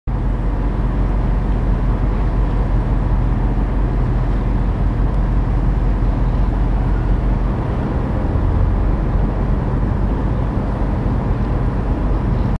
C0062 SAMYANG 85mm F1 8 FHD120p SteadyShot OFF
*팝코넷에서 SAMYANG 85mm F1.8 ED UMC CS 렌즈로 촬영한 신제품 기능 테스트 영상입니다.